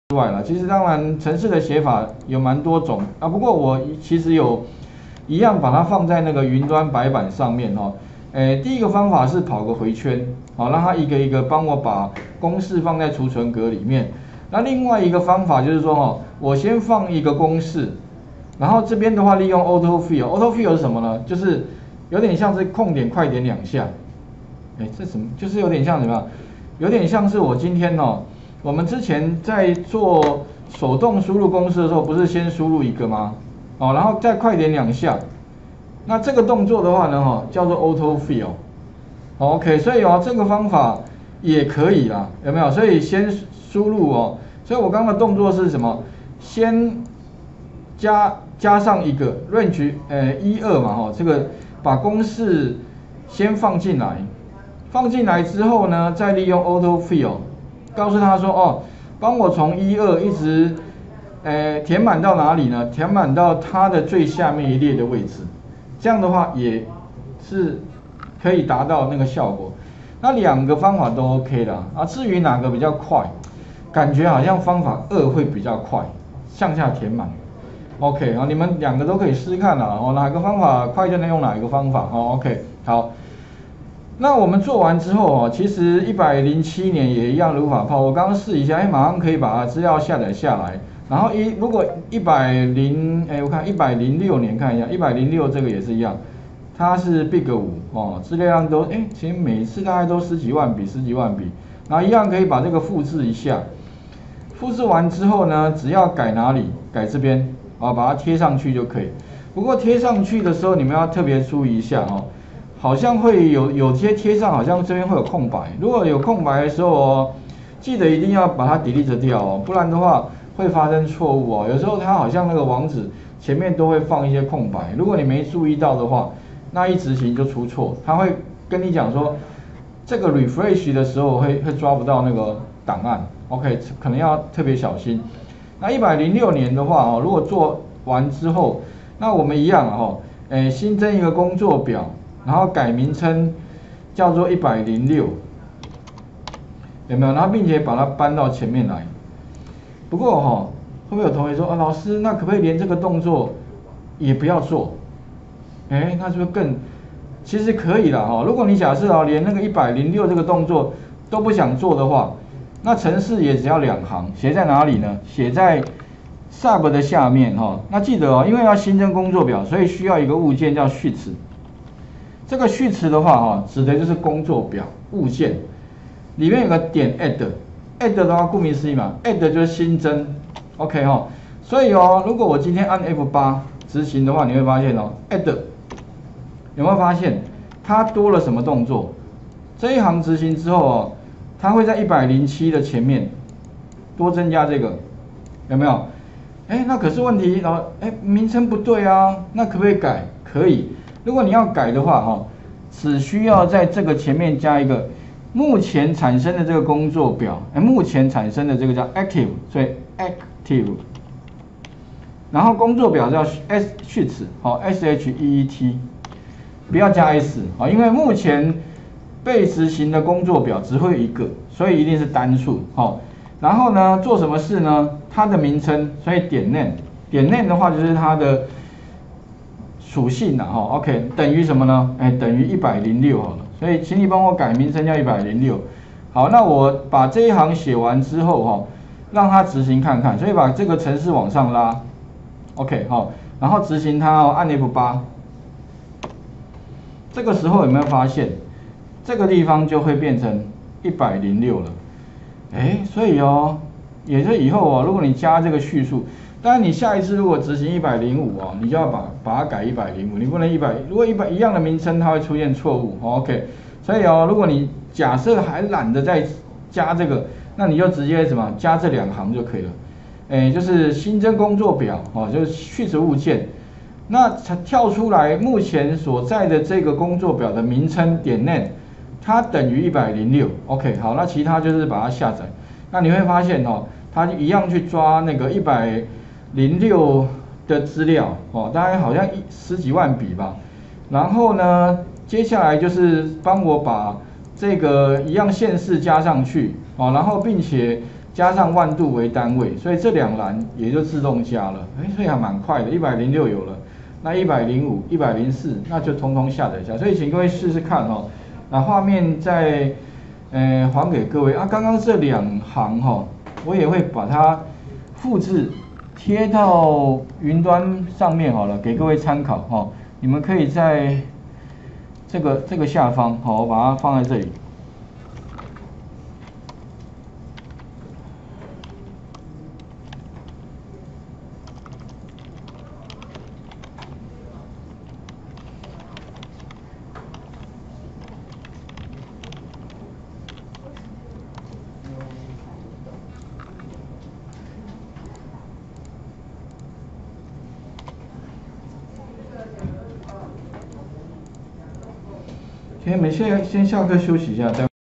05 新增工作表與重新命名
2022文獻數位化編輯與應用第4次上課錄影

01_重點回顧與樞紐分析
02_下載110年與109年資料說明
03_用QueryTables物件下載台電資料
04_下載程式與插入欄與公式
05_新增工作表與重新命名
06_下載104到111年資料
07_繳交作業到雲端與填寫座位表

吳老師  111/10/13